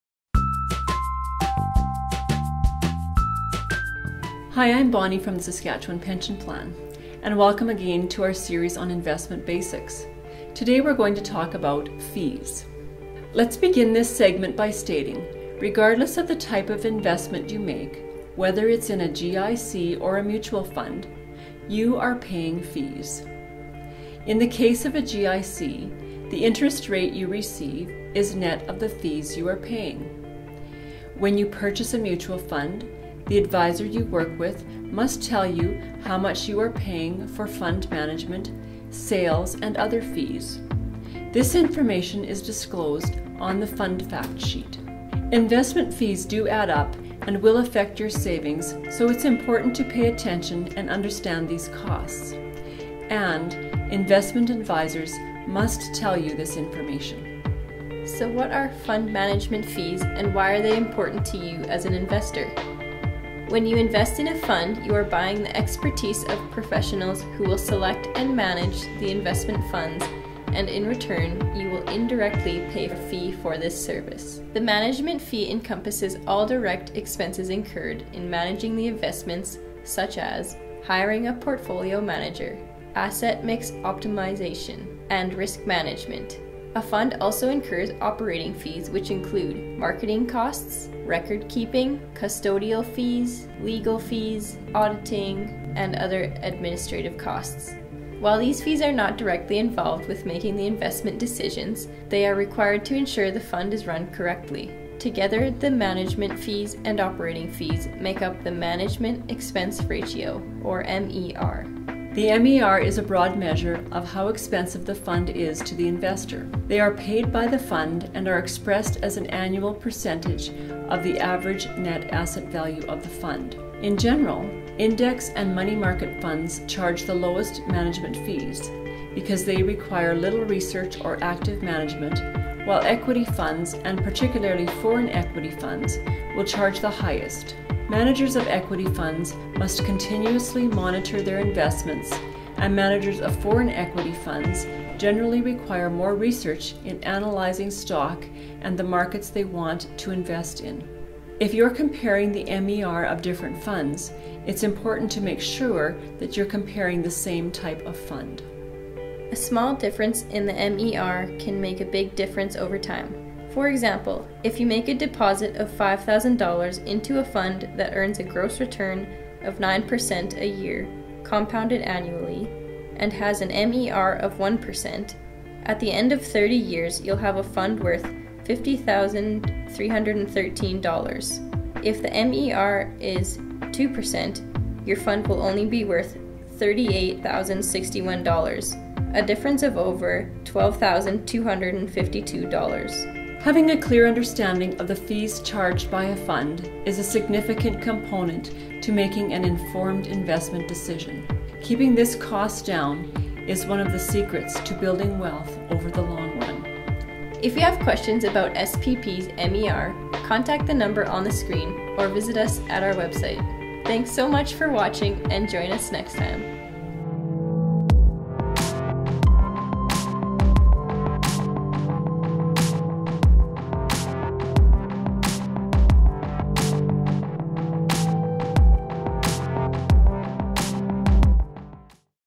Investment fees || SPP
Why should investors be concerned about fees?  How much of a difference do fees really make when saving for the future?  Get the straight facts in this video by SPP.

Stay in-the-know!

Video produced and filmed by: Saskatchewan Pension Plan